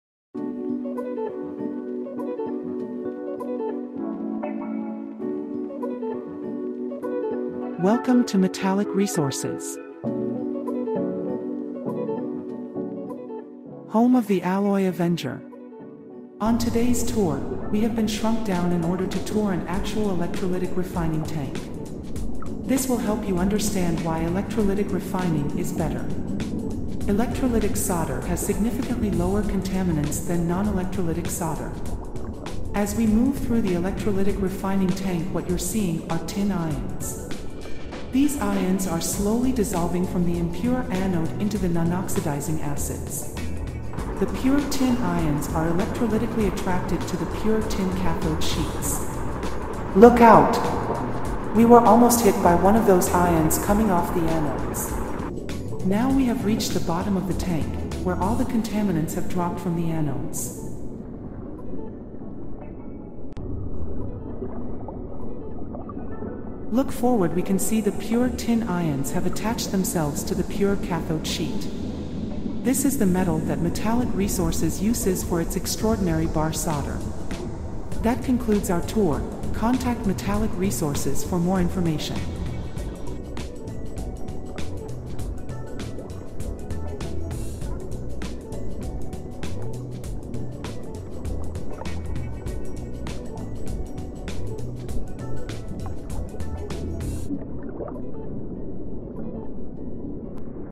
The Alloy Avenger
Welcome to Metallic Resources. Home of The Alloy Avenger.  On today's tour, we have been shrunk down in order to tour an actual Electrolytic Refining tank. This will help you understand why electrolytic refining is better.